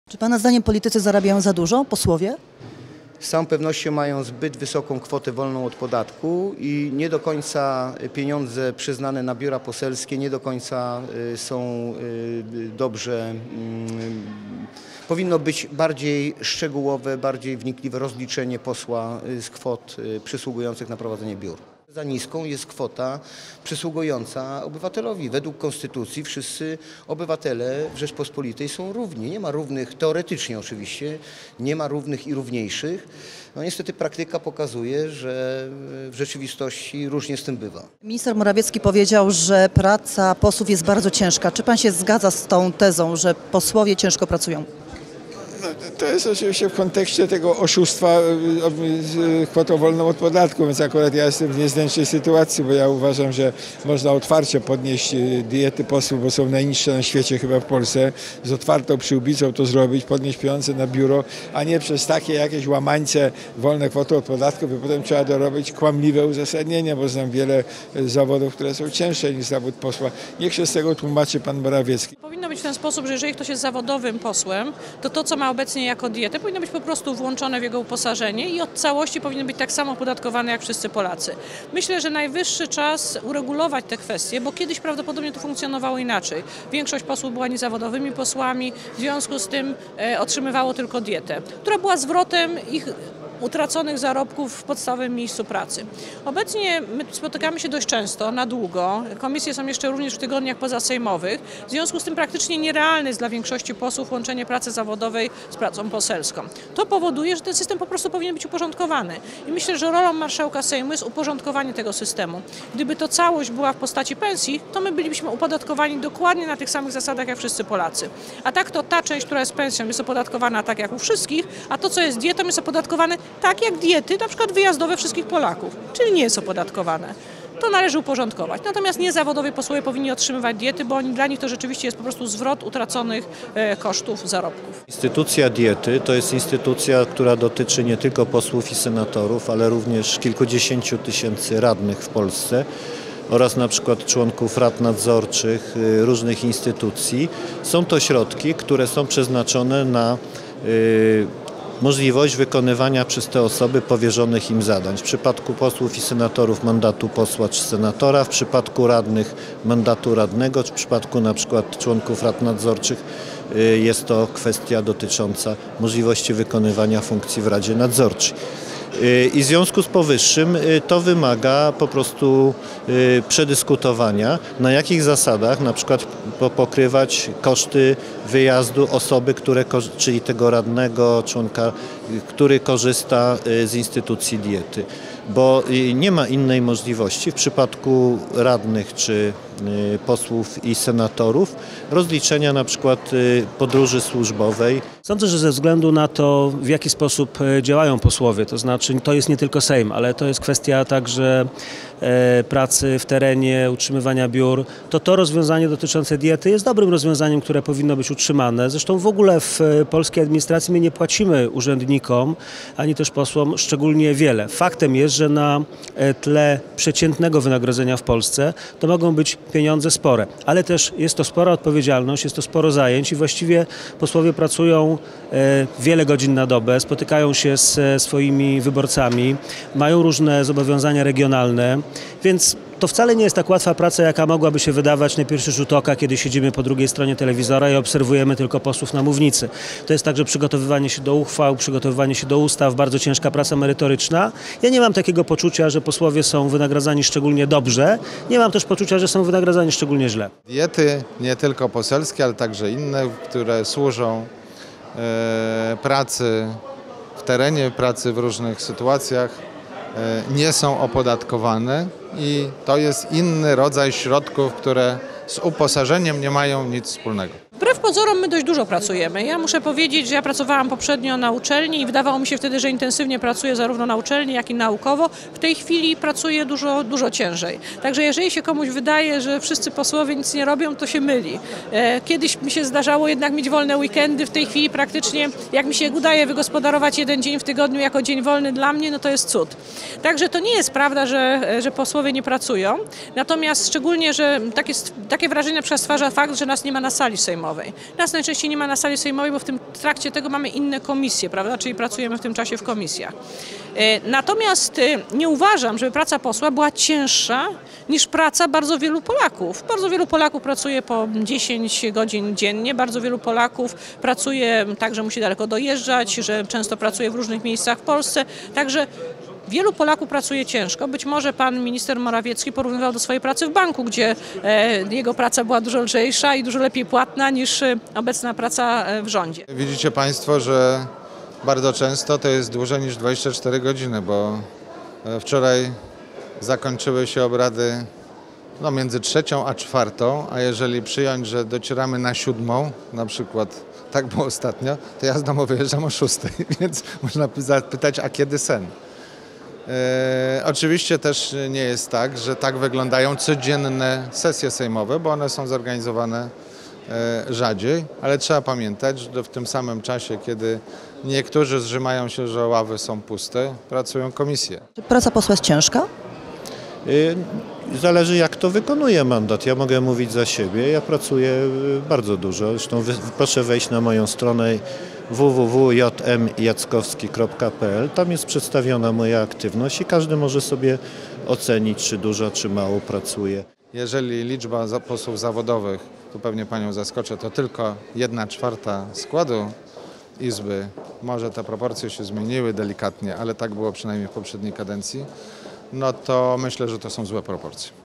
Zapytaliśmy posłów czy ich praca jest trudna i wyczerpująca
Zapytaliśmy polskich posłów o to czy uważają swoją pracę za trudną i wyczerpującą. Odpowiedzi mogą zaskakiwać. Posłowie opozycji ocenili też zmiany dokonane przez rząd w kwestii kwoty wolnej od podatku.